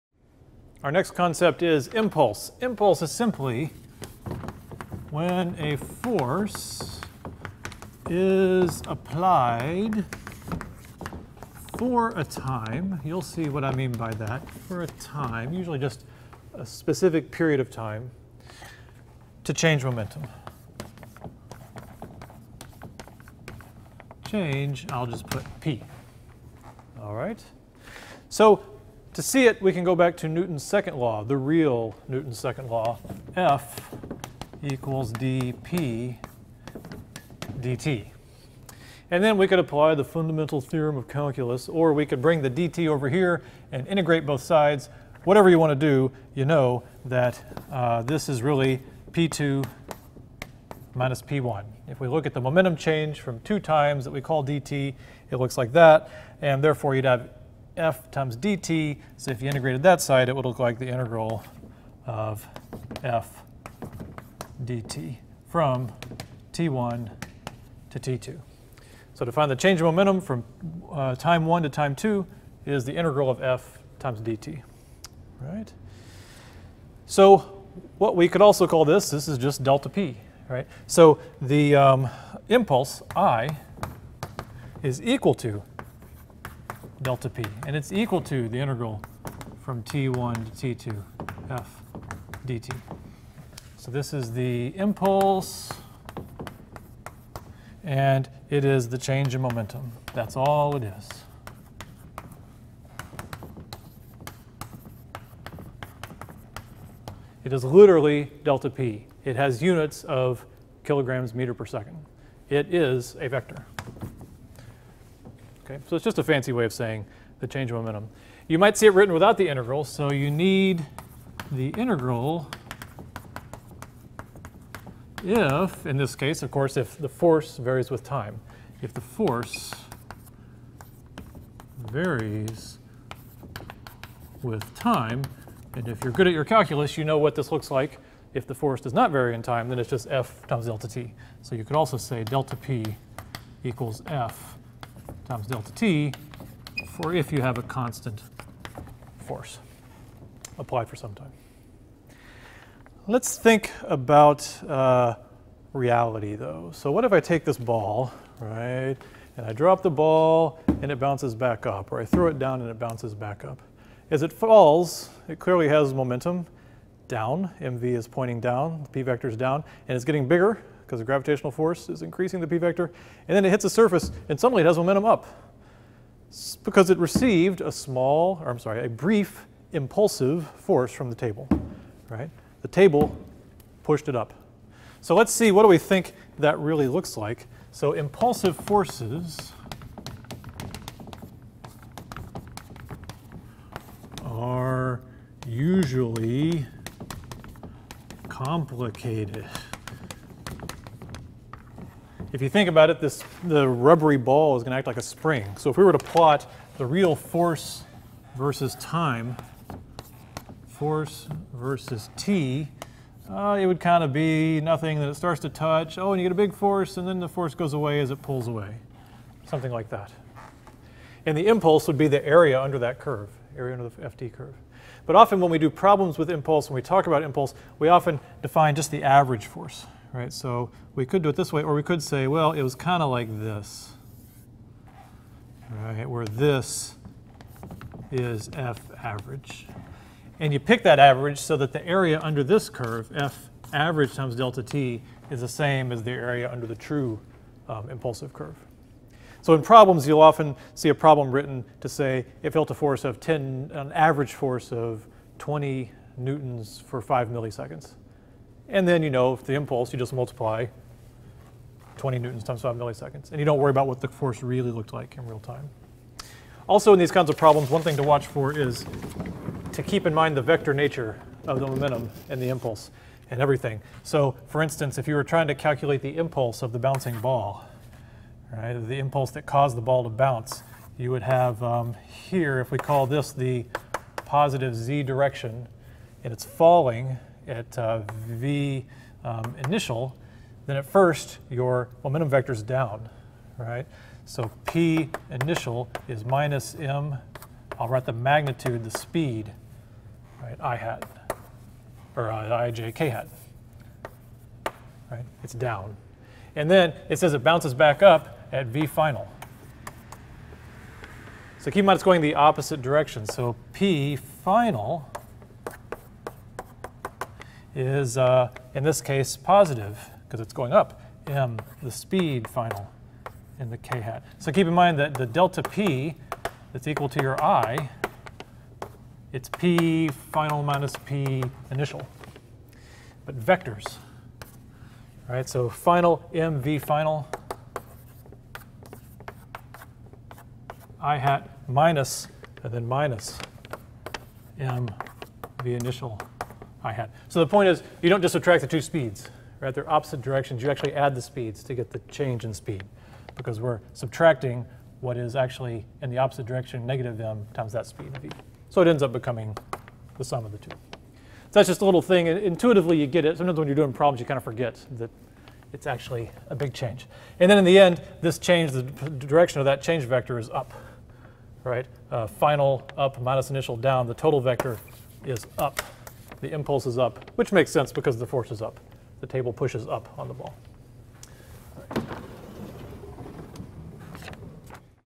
Momentum 5 - Impulse
We consider the brief forces that occur between objects when they strike each other and change momentum.